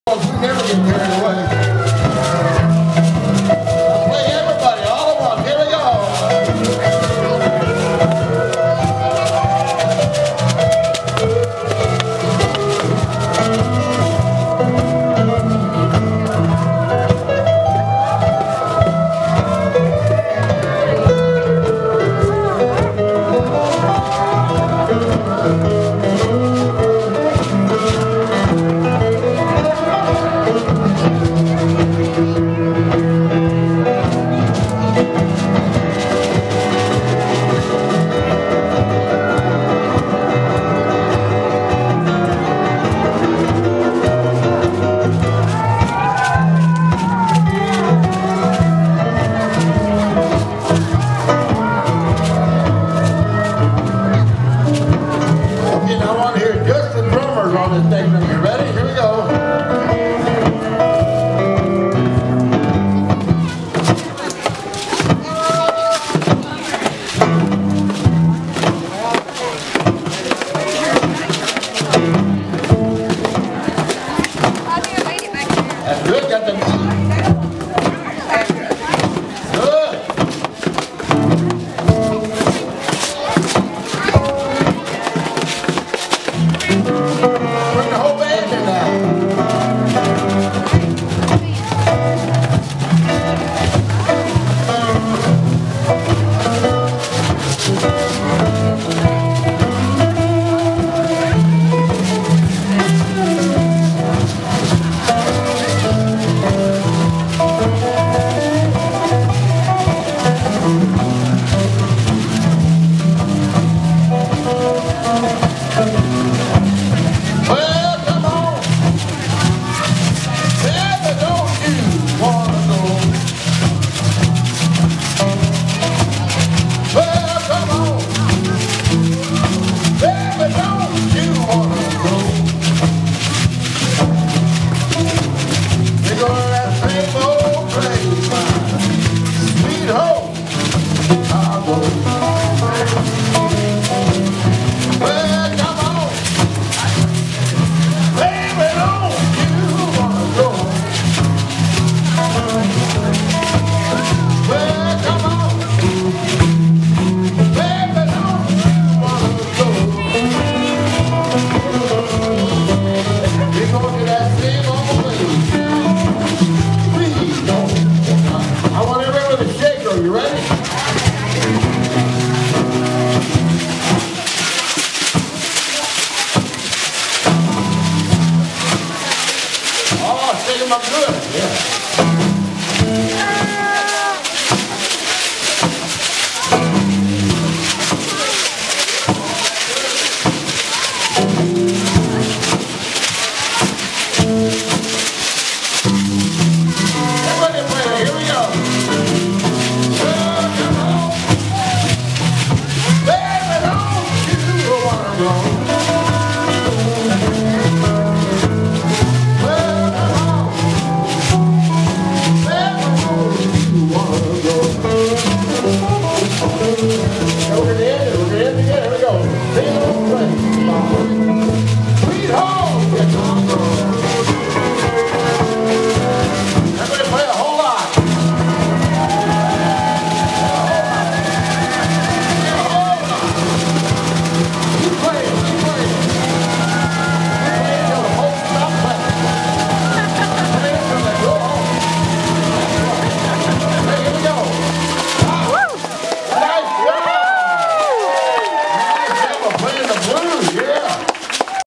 Making Music with Trash with Microwave Dave
FLPL kicks off Handy Week 2015 with Microwave Dave teaching the kids the blues on his cigar box guitar. Thanks to Florence Recycle for helping the kids create instruments from recycled materials.